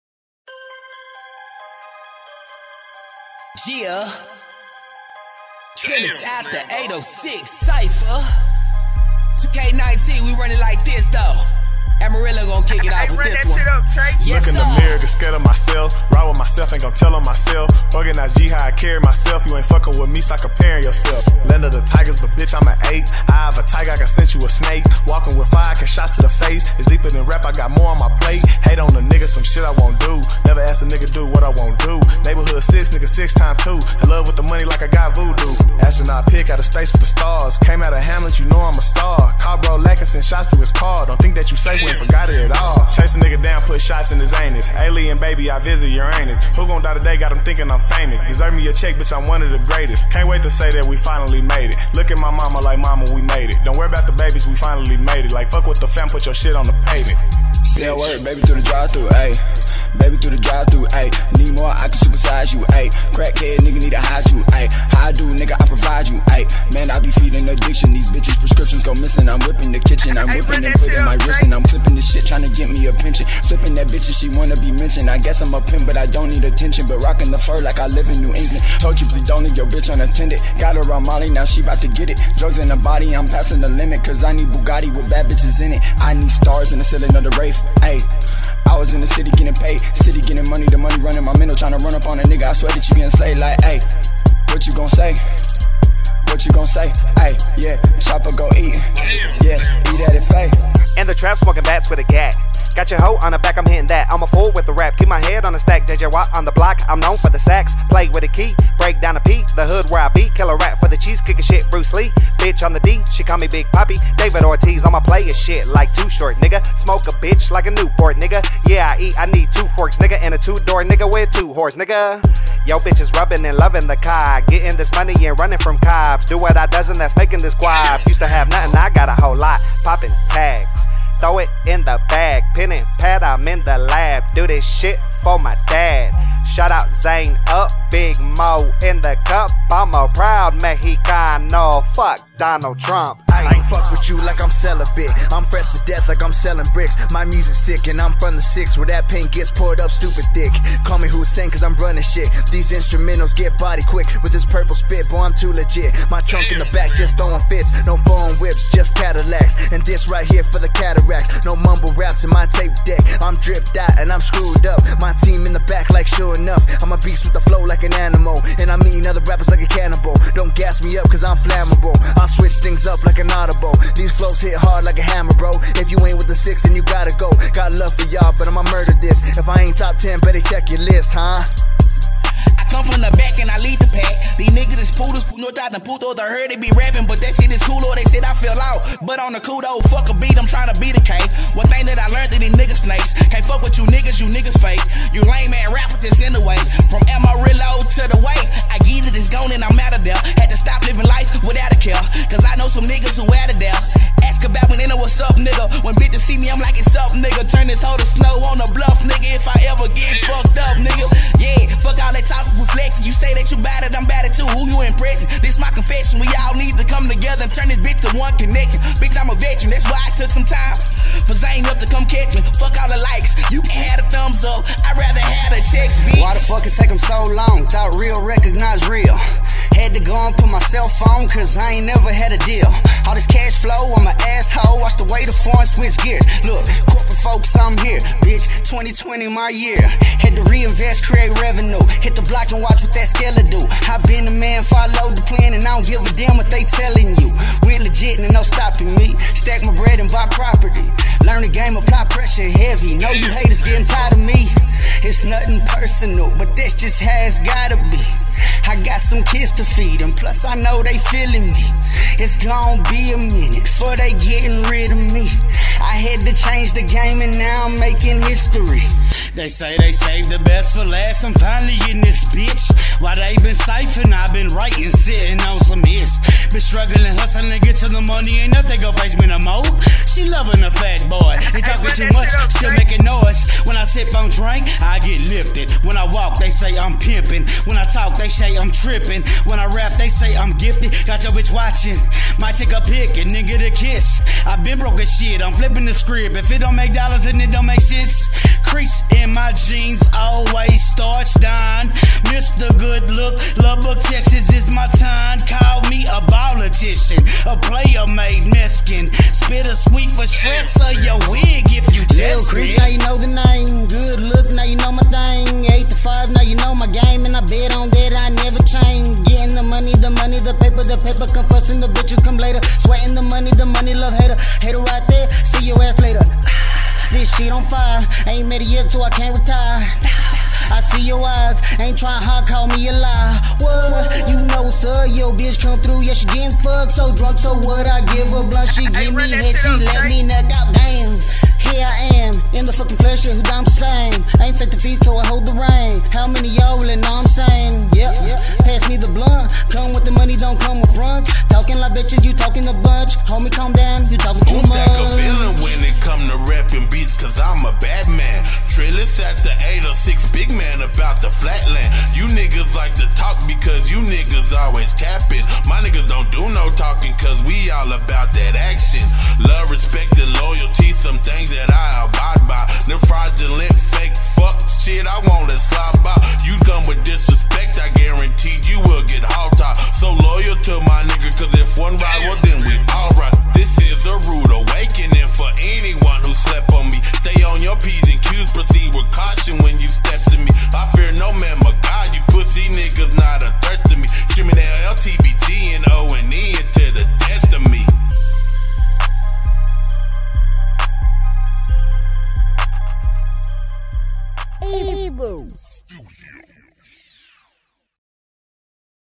West Texas Hub City Cypher (Shot by ZaneUp)
Eyez Wide Open Filmz
Hire @...

EWO presents Trillest out the 806 Hood Cypher..

Shot in Lubbock Texas..

Directed by Zane Up..

Lineup::
PJB
GOLDBAND LUCK
BIG PLAYA JC
ANIMAL
FREDDYROB
TOOCOLD
JAY FRICTION
LIL CREEPS
BOONSTAXXZ